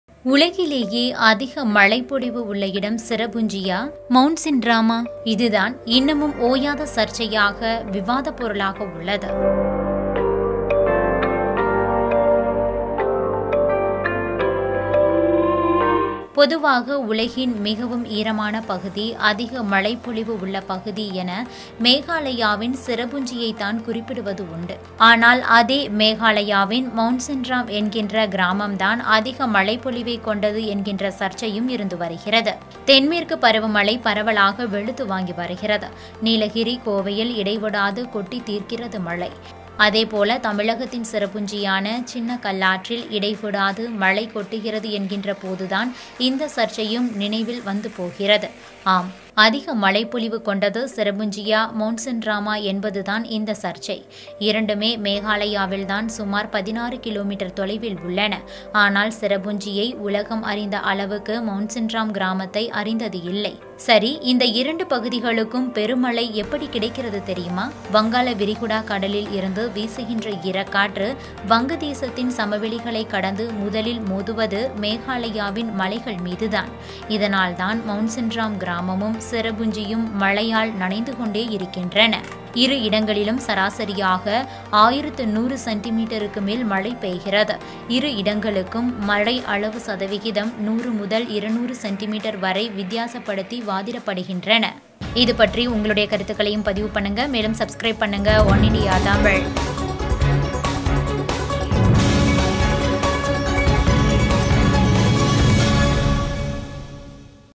உலகிலேயே அதிக மழைப் பொழிவு உள்ள இடம் சிரபுஞ்சியா?..மெளசின்ராமா?- வீடியோ
உலகிலேயே அதிக மழைப் பொழிவு உள்ள இடம் சிரபுஞ்சியா? மெளசின்ராமா? என்பது இன்னமும்

ஓயாத சர்ச்சையாக விவாதப் பொருளாகவே உள்ளது. பொதுவாக உலகின் மிகவும் ஈரமான பகுதி,

அதிக மழைப் பொழிவு உள்ள பகுதி என மேகாலயாவின் சிரபுஞ்சியைத்தான் குறிப்பிடுவது உண்டு.

ஆனால் அதே மேகாலாயாவின் மெளசின்ராம் என்கிற கிராமம்தான் அதிக மழைப் பொழிவைக்

கொண்டது என்கிற சர்ச்சையும் இருந்து வருகிறது.

Cherrapunji had top first position of wettest place in India. But some

observations argued that Mawsynram (nearby Cherrapunji) is received the

wettest place.